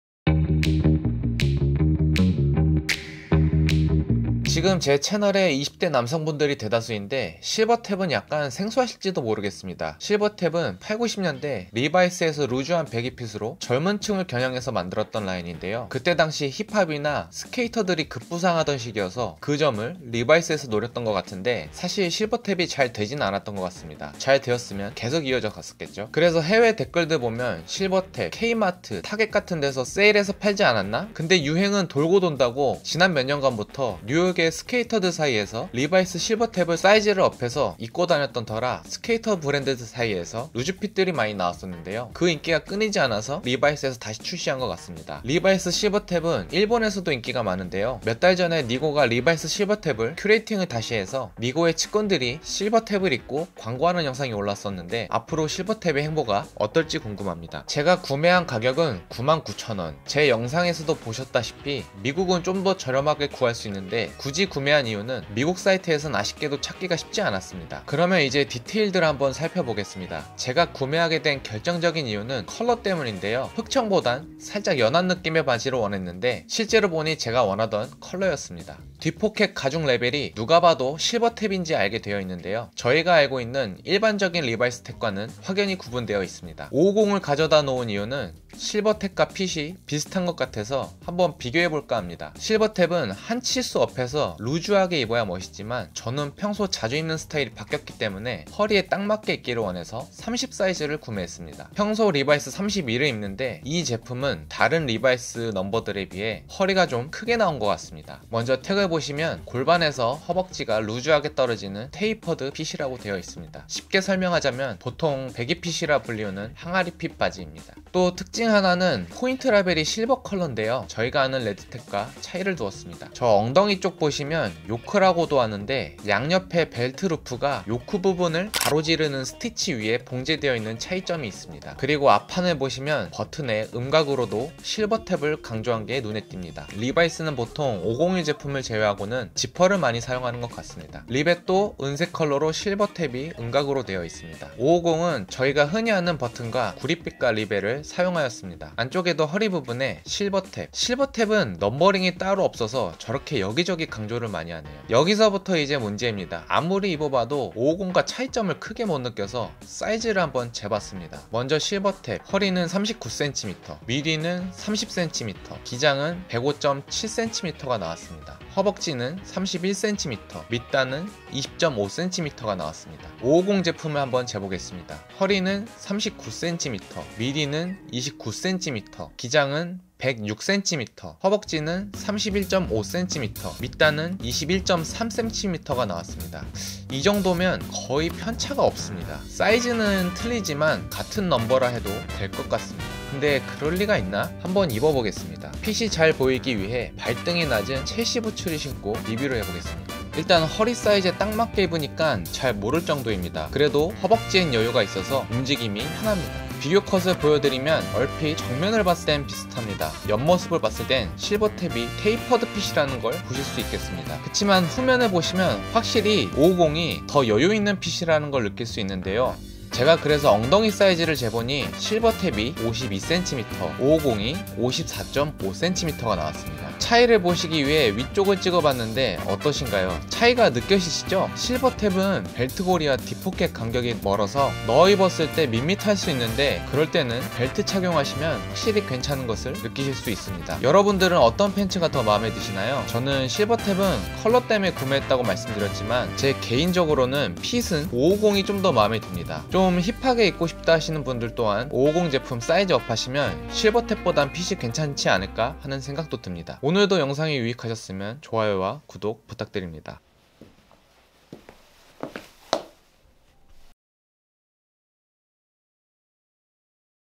리바이스 실버탭 vs 리바이스 550 (둘 중 루즈 핏을 찾는다면..)
얼마 전에 약속시간이 떠서 리바이스 매장에 들어갔더니 실버 탭에 제가 구매를 원하던 컬러가 있어서 구매를 해봤습니다. 근데 리바이스 550과 핏이 비슷해서 샅샅이 리뷰를 해보았습니다.

리바이스 실버 탭 루스 핏 30x32
Levi's Silver tab
리바이스 550 32x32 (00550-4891)
Levi's 550

감사합니다!